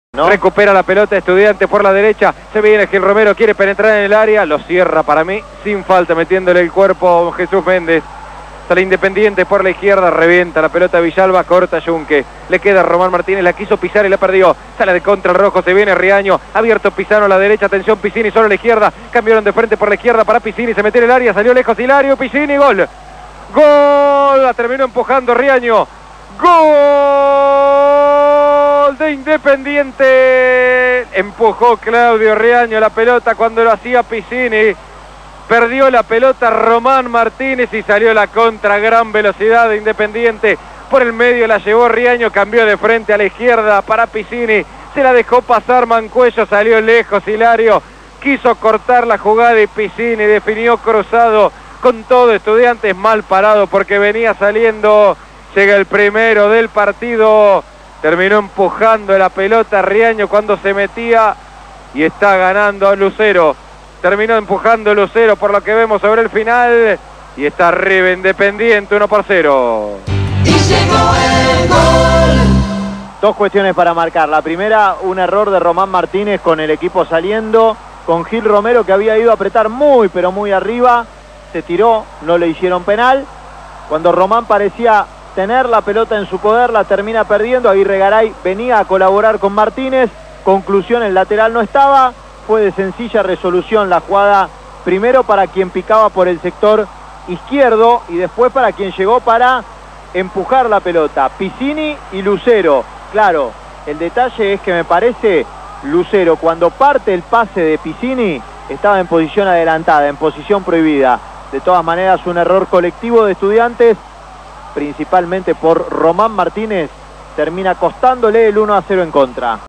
Estudiantes 0 Independiente 1 (Relato Federico Serra) Torneo de Verano 2015 (15/1/2015)
El gol del partido con relato de Federico Serra FM La redonda 100.3